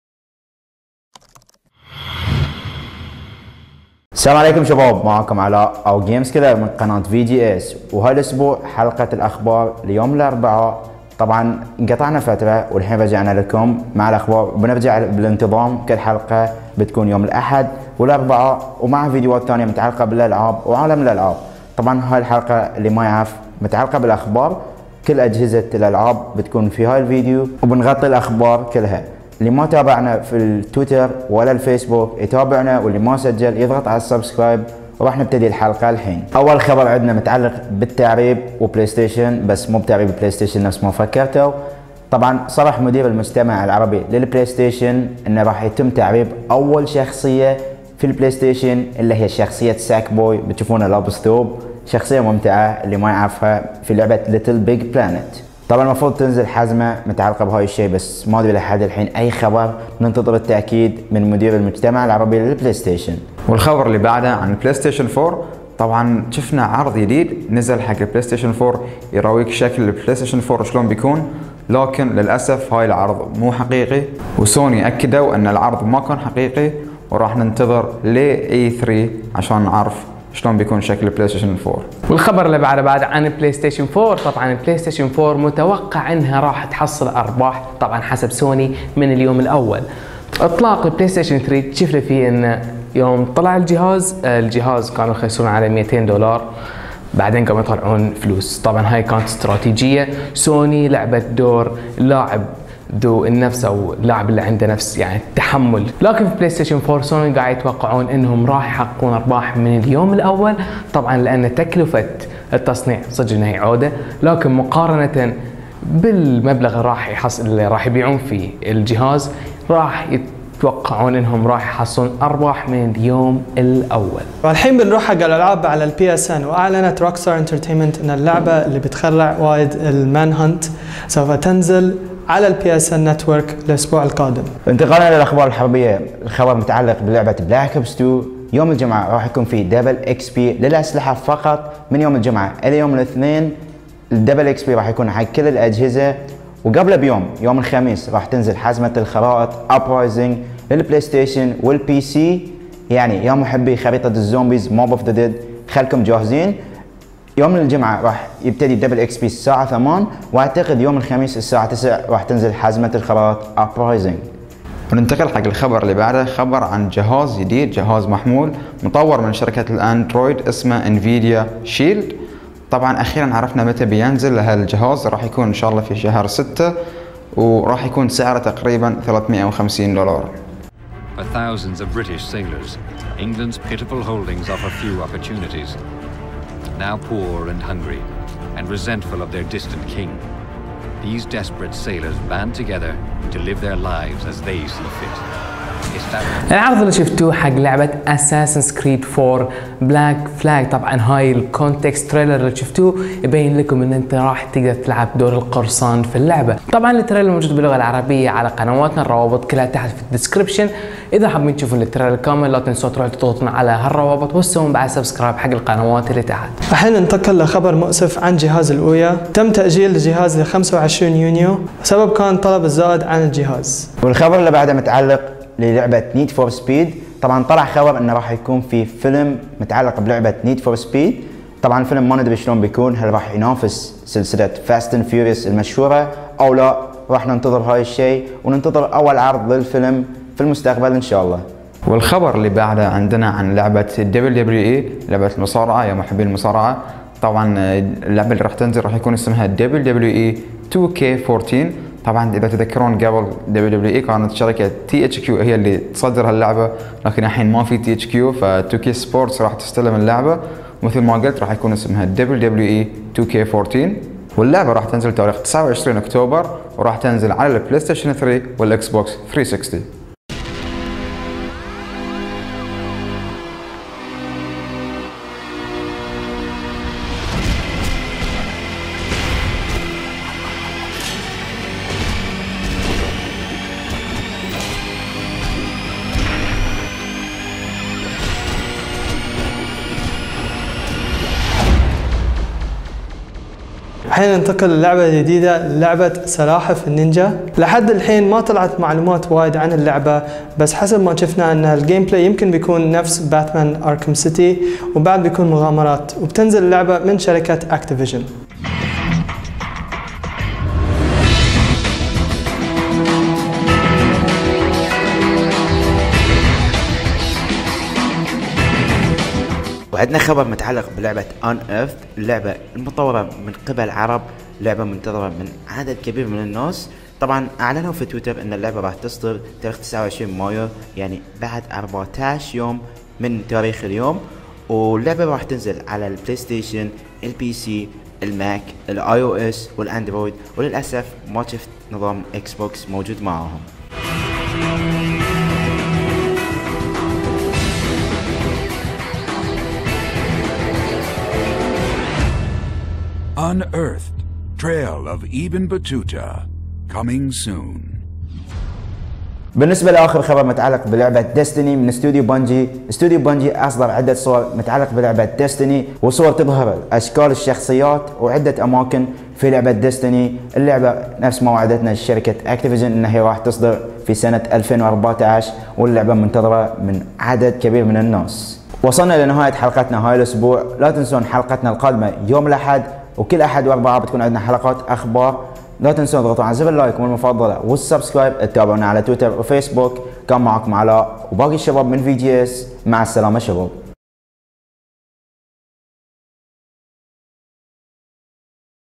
محطة أخبار الألعاب: اعلان سوني الكاذب
• شكر خاص لكل شخص ضغط على زر اللايك والمفضلة

Playstation 4 reveal fake
Call of duty: ghosts leaked